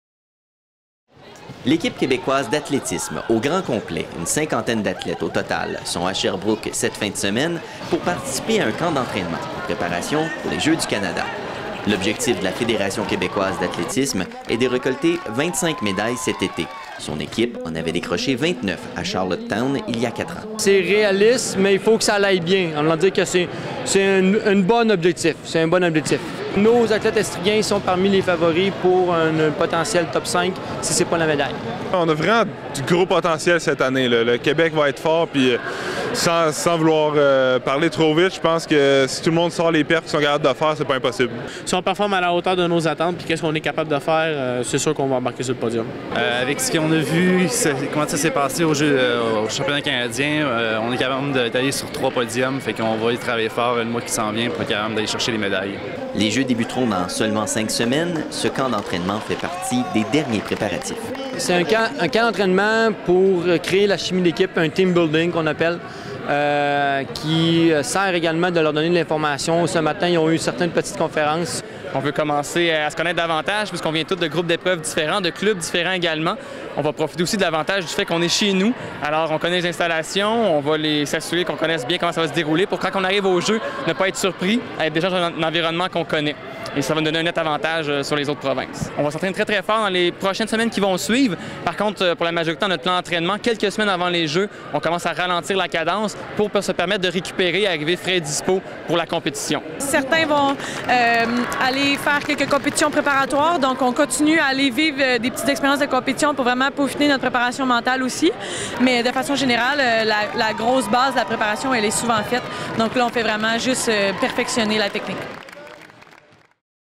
Des athlètes de Sherbrooke et du Québec en route pour les Jeux du Canada
Les membres de l'équipe québécoise d'athlétisme se fixe un objectif de 25 médailles lors des Jeux du Canada, qui se tiendront à Sherbrooke dans cinq semaines.